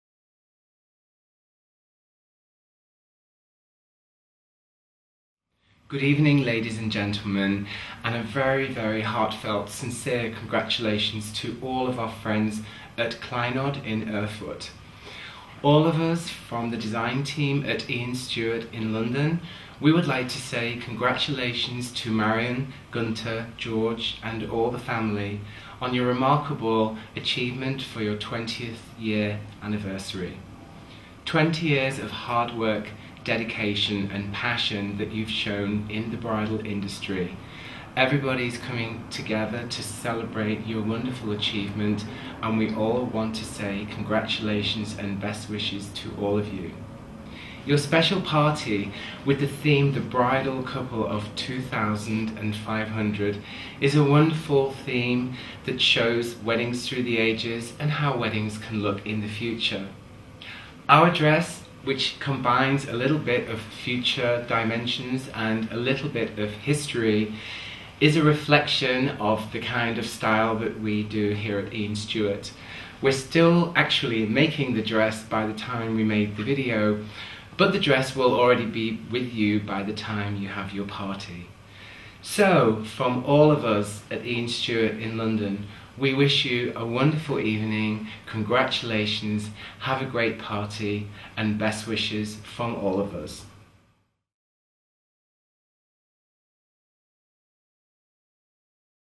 Ian Stuart an Kleinod
UK Brautkleiddesigner Ian Stuart grüßt Hochzeitsausstatter Kleinod und stellt sein "Brautkleid 2500 " vor